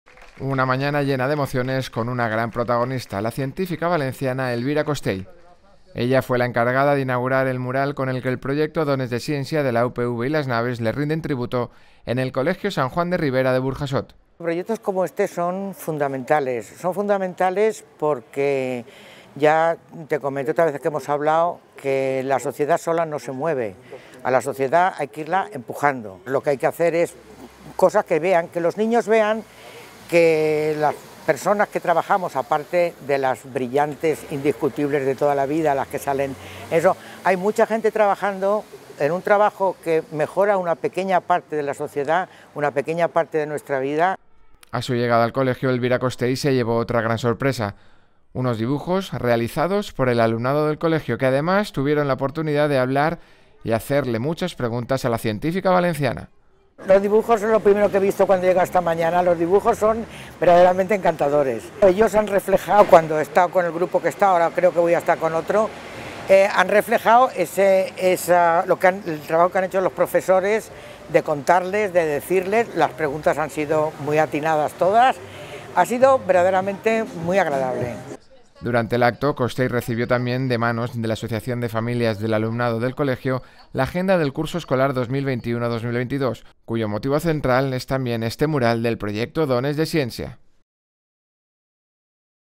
Dones de ciència: Elvira Costell (lengua de signos)
El proyecto Dones de Ciència de la Universitat Politècnica de València (UPV) y el centro de innovación Las Naves del Ayuntamiento de València suma desde hoy un nuevo mural. Situado en el CEIP San Juan de Ribera de Burjassot, rinde homenaje a la científica valenciana Elvira Costell. Y lo hace con la firma de la artista catalana Btoy.